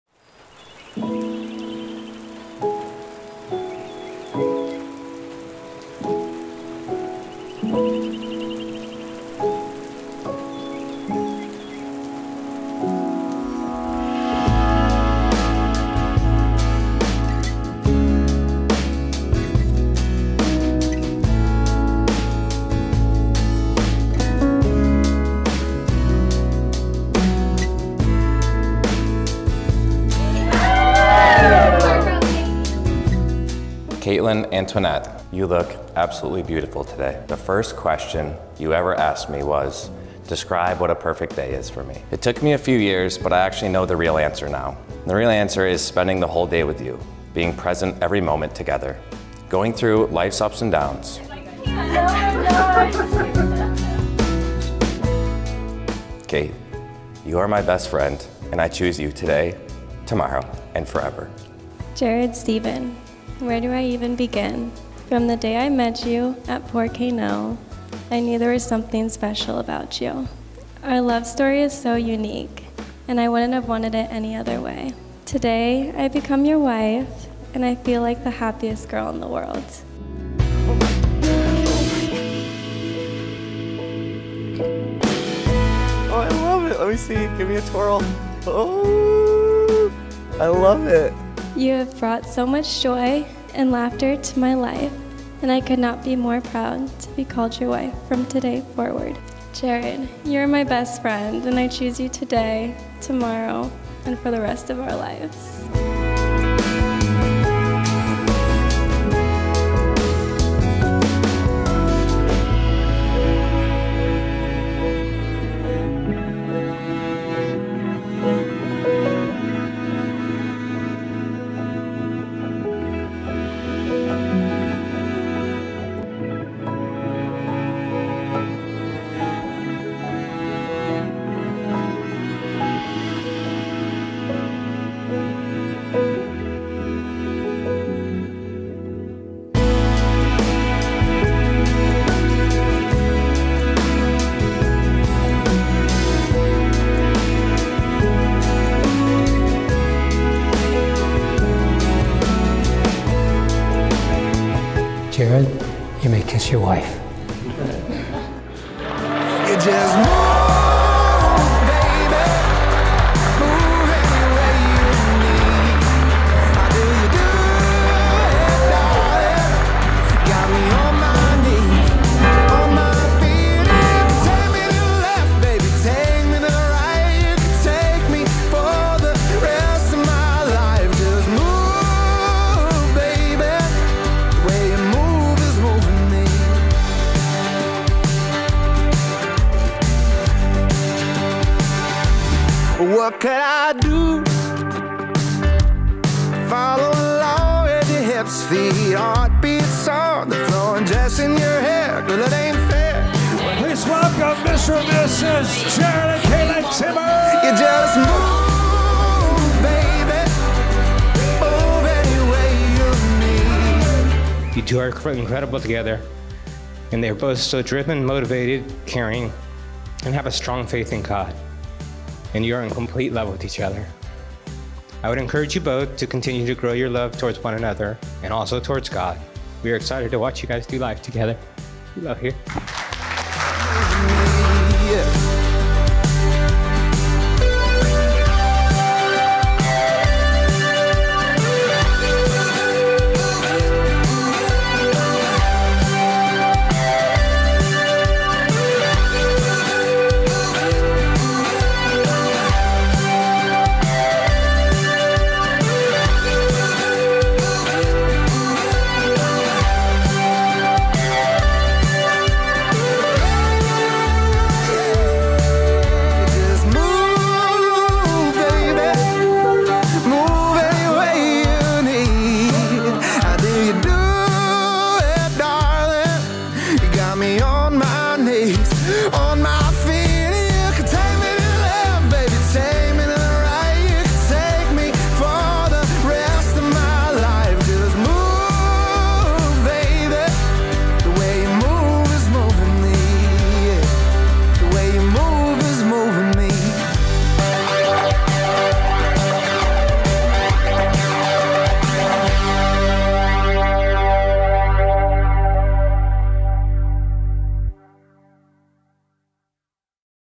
Abernethy Center | Oregon City Wedding - Caitlyn + Jared's Highlight Reel [12.30.22]
In this video, Candy Glass Productions shot a beautiful wedding for Melissa + Adam on 12.30.22 at Abernethy Center in Oregon City, Oregon.